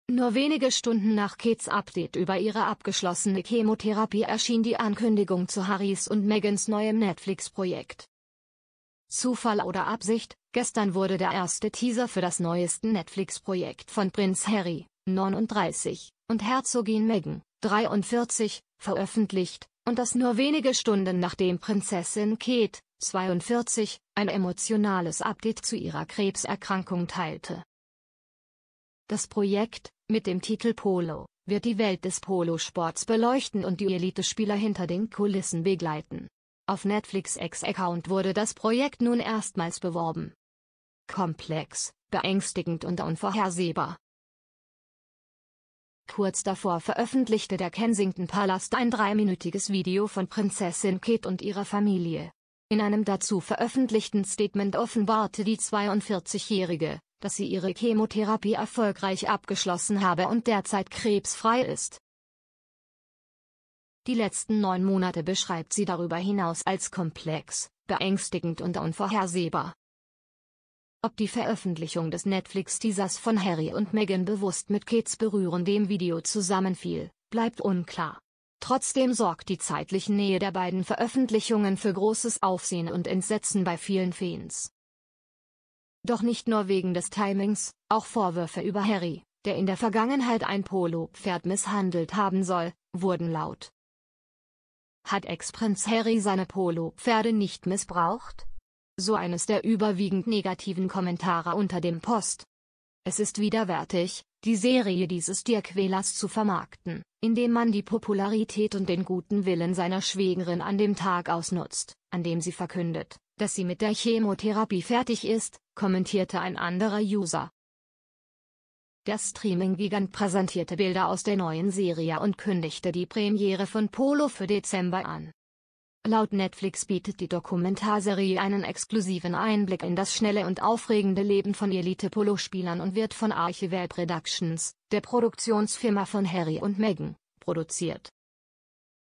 Kurz nach Kates Video Meghan & Harry machen alle sauer
Nur wenige Stunden nach Kates Update über ihre abgeschlossene Chemotherapie erschien die Ankündigung zu Harrys und Meghans neuem Netflix-Projekt.